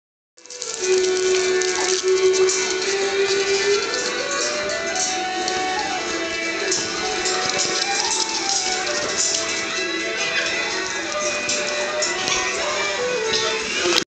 Ferret Leg Climb
Erma climbs legs when she wants treats or she gets tired and wants to cuddle.